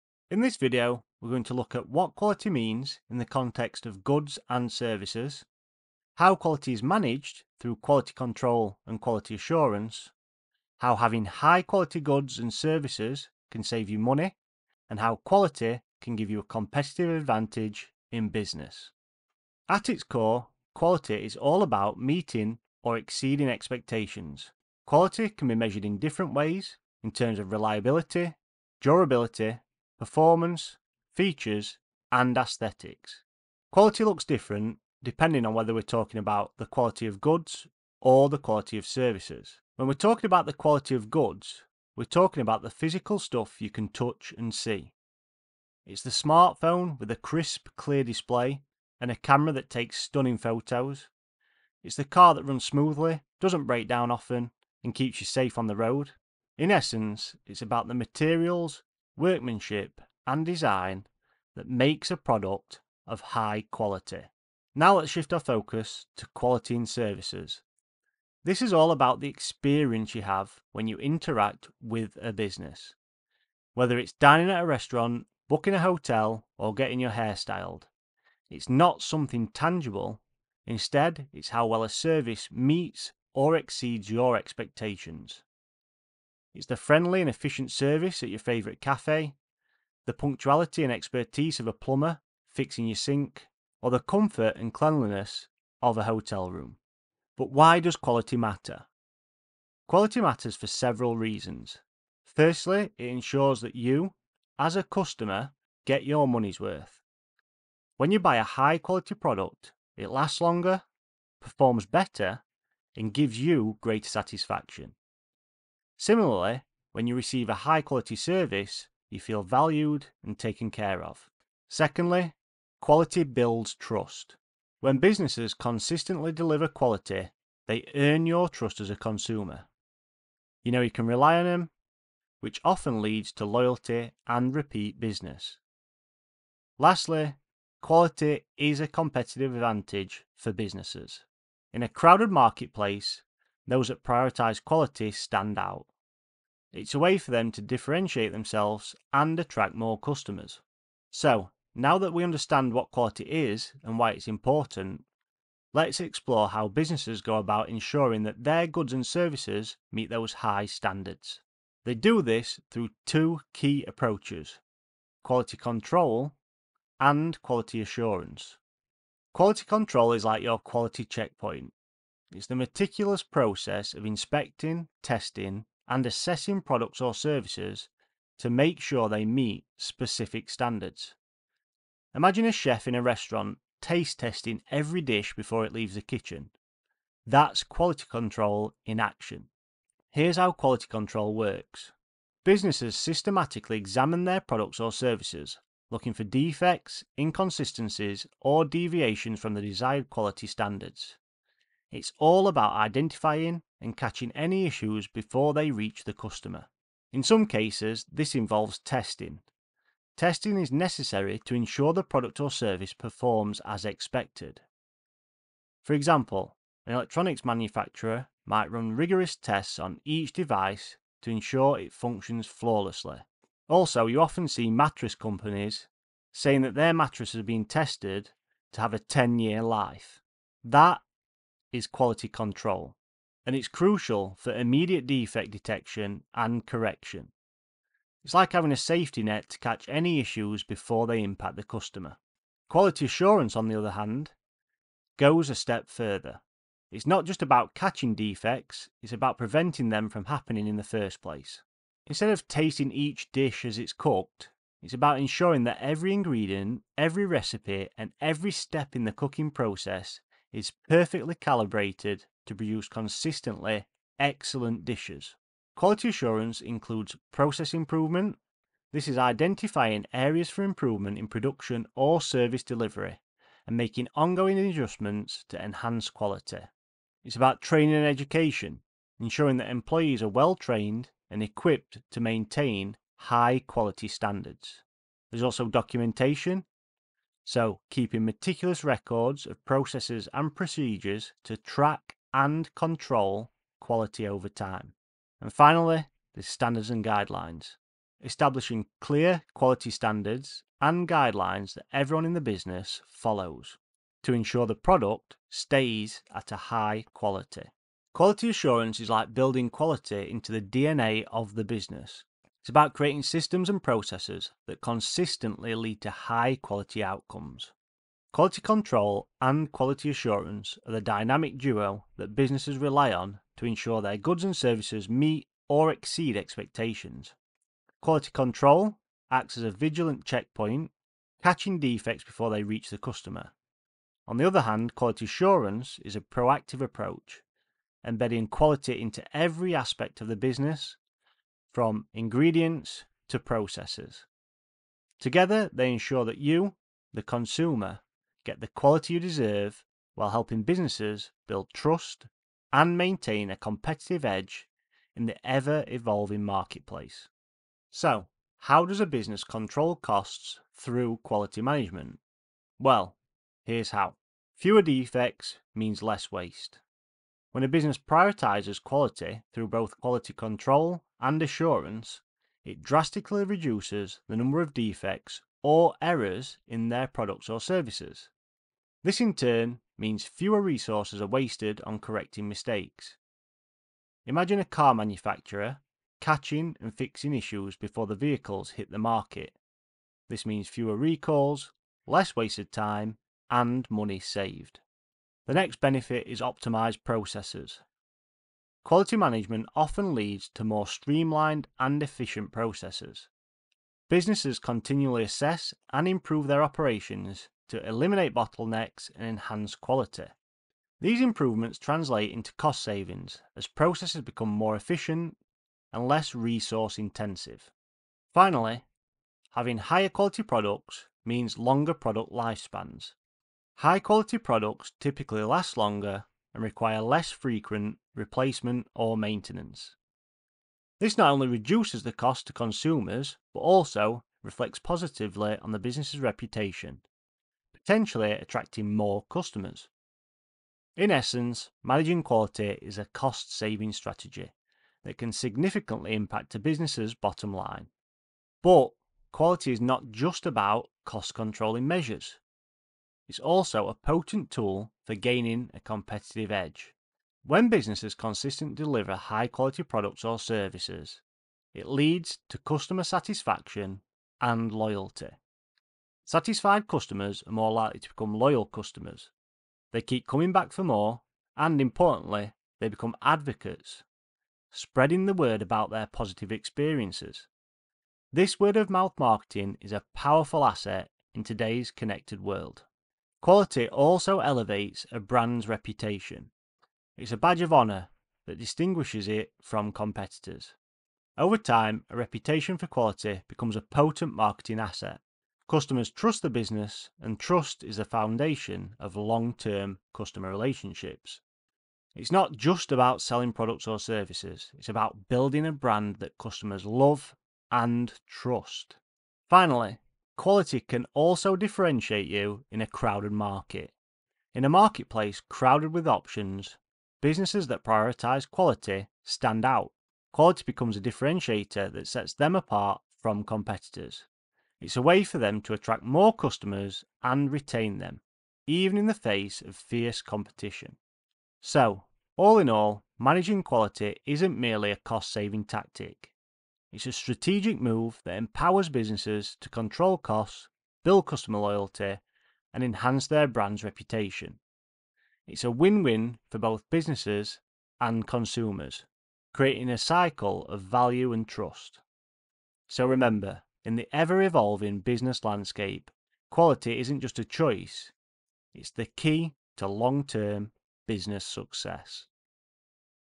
Managing Quality Explained | Quality Control & Quality Assurance
Watch this video if you want to learn about how businesses manage quality through quality control and quality assurance. Plus how quality management can save a business money and give them a competitive advantage.

Chapters:
00:19 What is quality?
00:39 How to measure the quality of goods
01:04 How to measure the quality of services
01:38 Why does quality matter?
02:49 What is quality control?
04:02 What is quality assurance?
05:58 How managing quality saves money
07:28 How managing quality is a competitive advantage